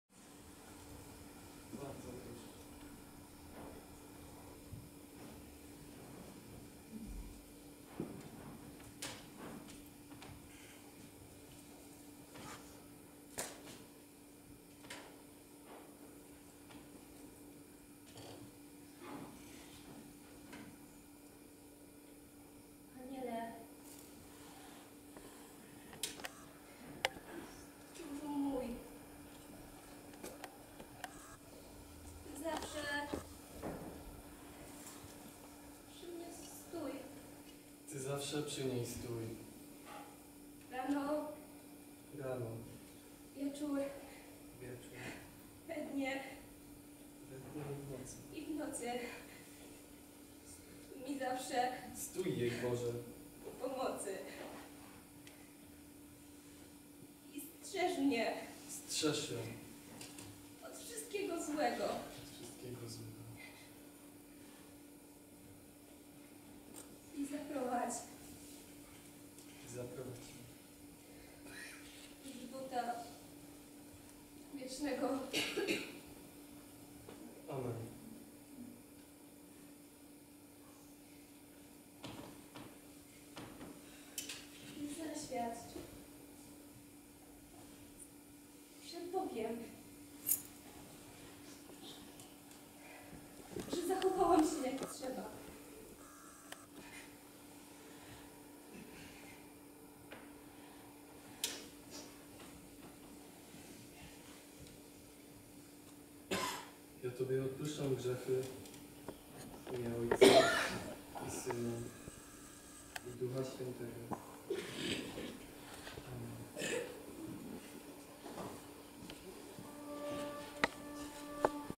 "Ja Danuta Siedzikówna INKA" w Teatrze Dzieci Zagłębia w Będzinie 012
Premiera spektaklu teatralnego "Ja, Danuta Siedzikówna INKA" w wykonaniu Kameralnego Teatru Małych Form KaTeMaFor, 25 marca 2017 r., autor filmu: Piotr Dudała